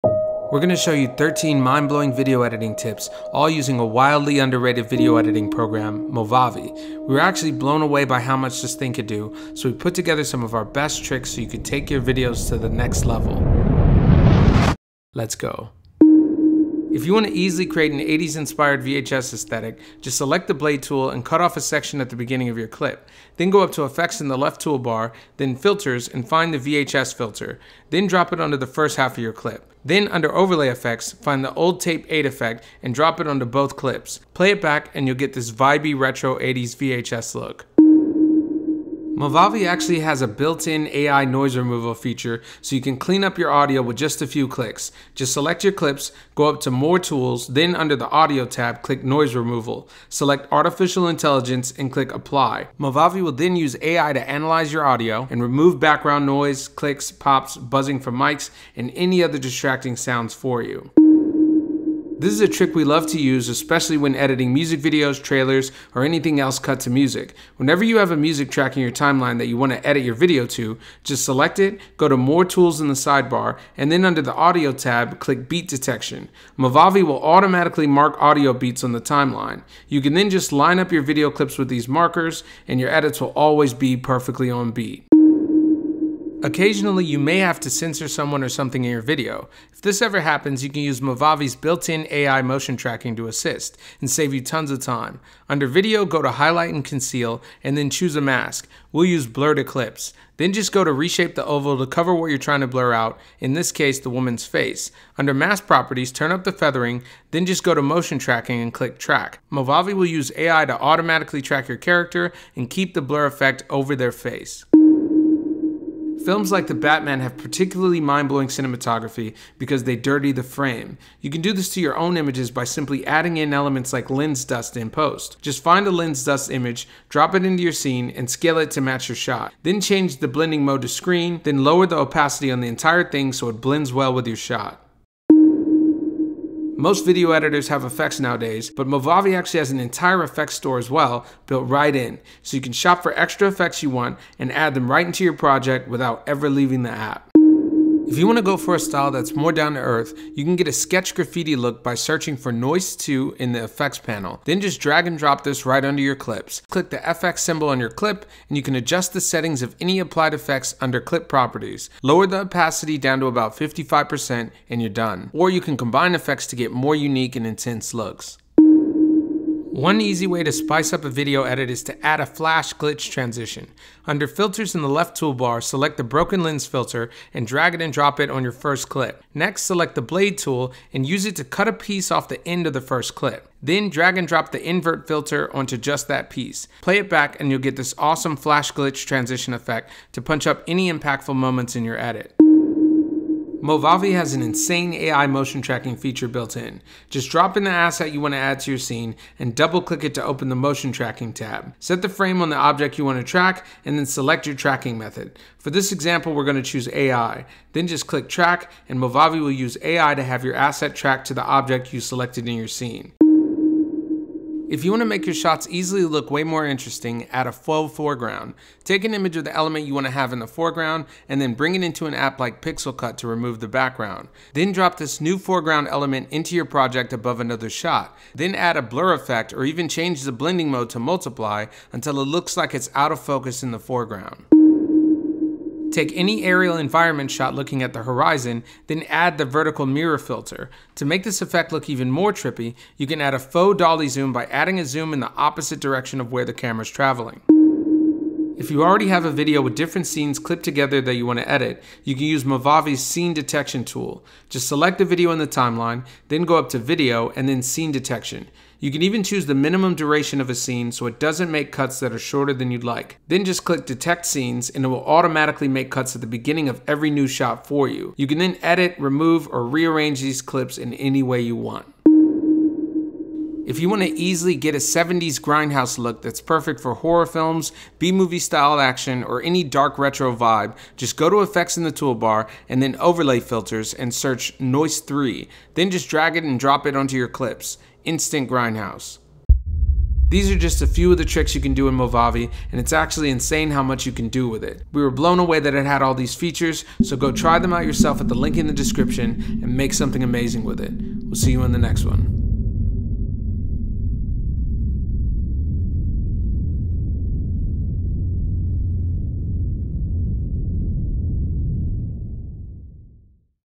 13 Mindblowing Video Editing Tips (Using Movavi?)
Creative video editing tips for filmmakers and content creators to make mindblowing videos.

Most people have no idea CapCut for desktop even exists.

And even we were blown away by how much it can do.

00:00 Intro
00:16 80s VHS Aesthetic
00:41 AI Noise Removal
01:01 Beat Detection
01:25 Censor Tracking
01:54 Dirty The Frame
02:15 Effects Shopping
02:27 Sketch Graffiti
02:48 Flash Glitch Transition
03:15 AI Motion Tracking
03:33 Faux Foreground
03:54 Mirrored Aerial
04:07 Scene Detection
04:32 Grindhouse Look

MOVAVI

All the sounds in this video are from our popular SINGULARITY Cinematic Sound Effects Library.